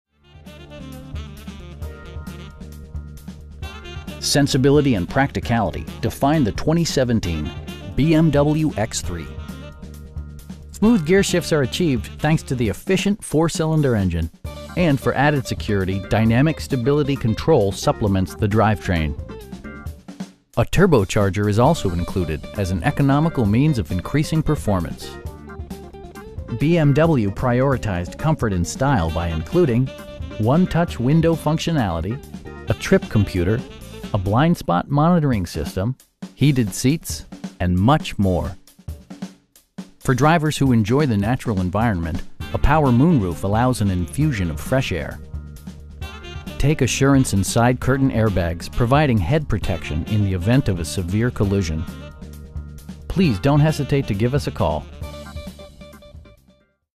2017 BMW X3 sDrive28i in Jacksonville, FL 32244
Sensibility and practicality define the 2017 BMW X3. Smooth gearshifts are achieved thanks to the efficient 4 cylinder engine, and for added security, dynamic Stability Control supplements the drivetrain. A turbocharger is also included as an economical means of increasing performance. BMW prioritized comfort and style by including: 1-touch window functionality, a trip computer, a blind spot monitoring system, heated seats, telescoping steering wheel, power door mirrors, and much more. For drivers who enjoy the natural environment, a power moon roof allows an infusion of fresh air. Take assurance in side curtain airbags, providing head protection in the event of a severe collision. You will have a pleasant shopping experience that is fun, informative, and never high pressured. Please don't hesitate to give us a call.